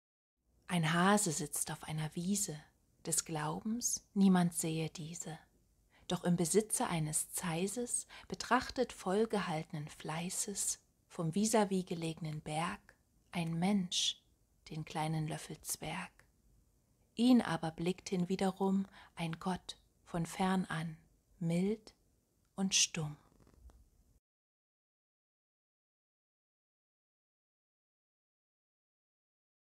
Christian Morgenstern ~ Vice versa 🐰
FROHE OSTERN! 🐰🐣🐑

Vice versa

Ein Hase sitzt auf einer Wiese,
des Glaubens, niemand sähe diese.
Doch im Besitze eines Zeißes,
betrachtet voll gehaltnen Fleißes
vom vis-à-vis gelegnen Berg
ein Mensch den kleinen Löffelzwerg.
Ihn aber blickt hinwiederum
ein Gott von fern an, mild und stumm.

(Christian Morgenstern, 1910)

Danke fürs Zuhören! 🙏